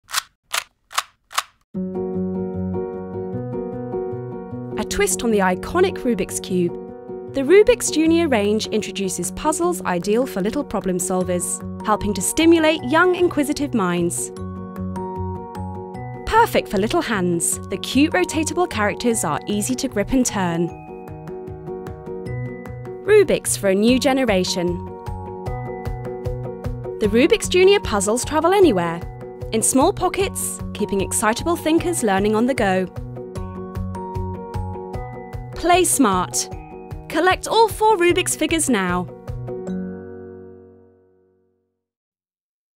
Rubik's Junior video
Watch the Rubik's Junior video to find out more about our Junior range. The ideal puzzles for little problems solvers.

for more exciting offers.